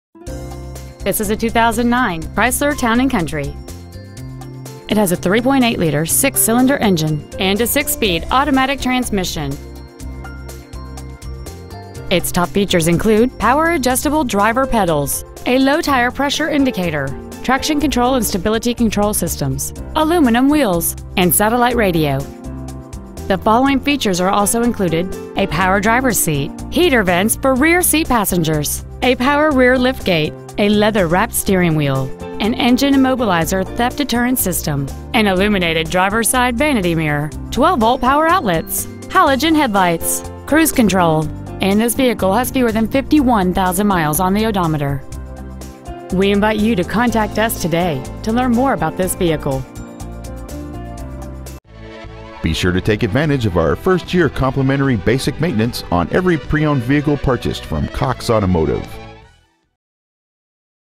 2009 Chrysler Town & Sarasota FL
Year : 2009

 Make : Chrysler

 Model : Town &

 Engine : gas v6 3.8l/231

 Trans . : automatic

 Exterior : red

 Miles : 50,698

 Interior : gray

 Cox Automotive

 2900 Cortez Road West

 2009 Chrysler Town & Bradenton FL